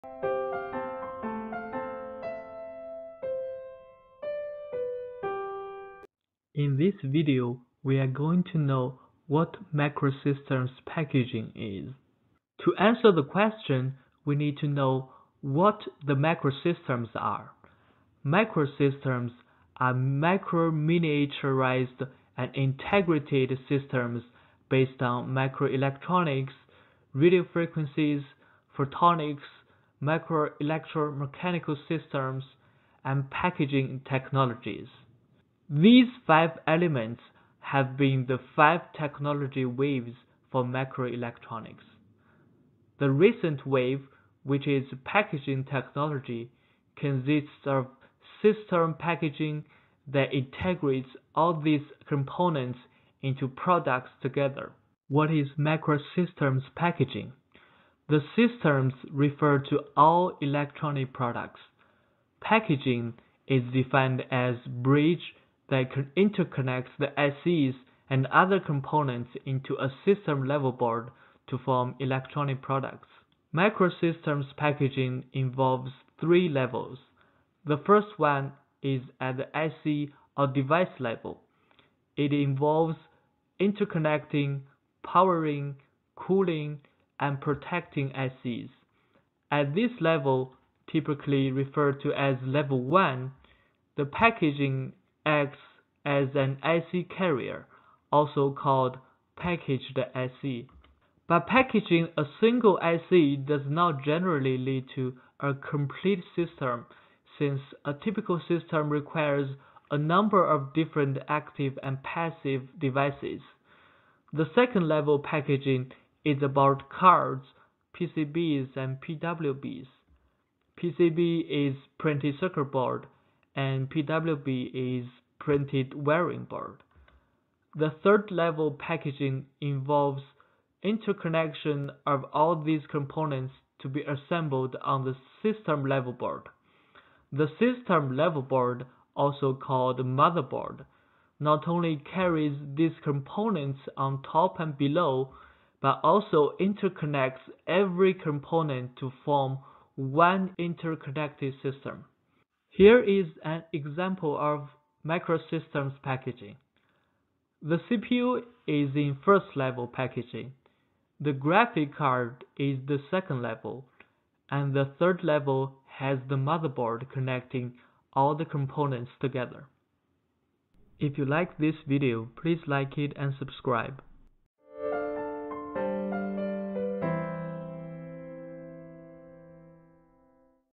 What Is Microsystems Packaging
What are microsystems, microelectronics packaging, levels of packaging? An example of computer breakdown shows the levels of packaging.